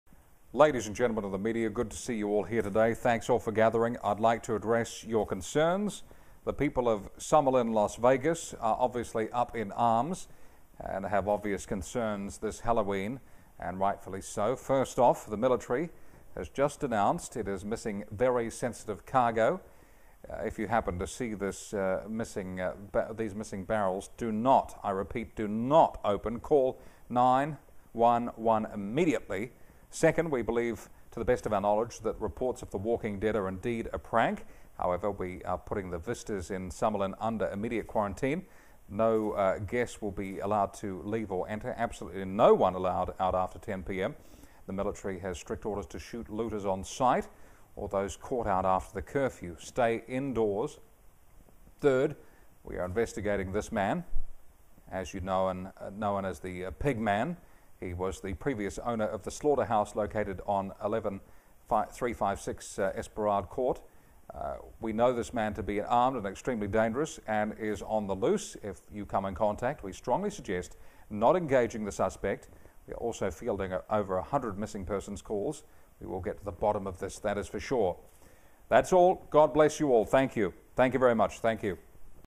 WE INTERUPT THIS PROGRAM - IMPORTANT PIGMAN NEWS UPDATE! HALLOWEEN NIGHT
This was a special news report halloween night about a mass murderor on the lose in Las Vegas. Also , about a missing Military Shipment and reports of the Walking  Dead